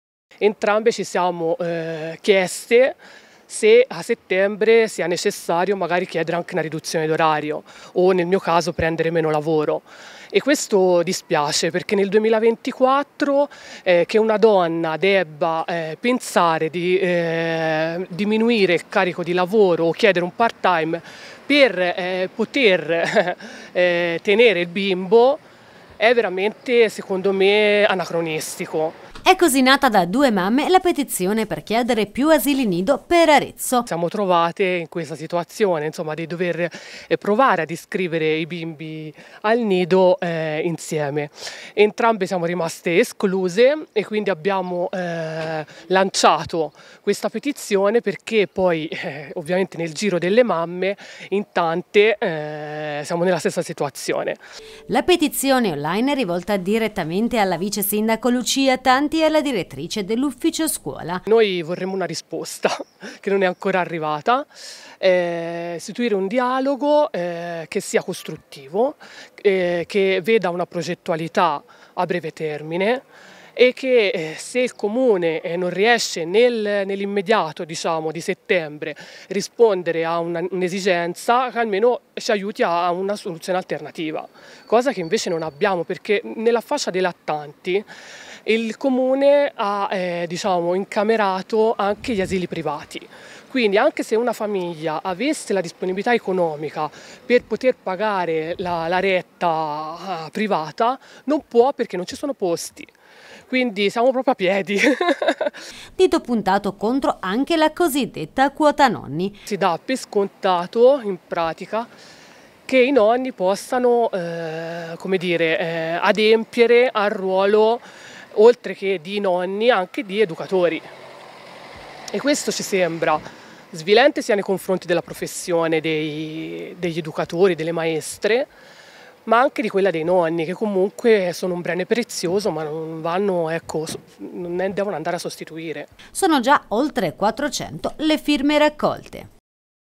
"Più posti nei nidi d'infanzia per i bambini di Arezzo", due mamme lanciano una petizione online
E' nata da due mamme la petizione per chiedere più asili nido per Arezzo.
La petizione è rivolta direttamente alla vicesindaco Lucia Tanti e alla direttrice dell'ufficio scuola.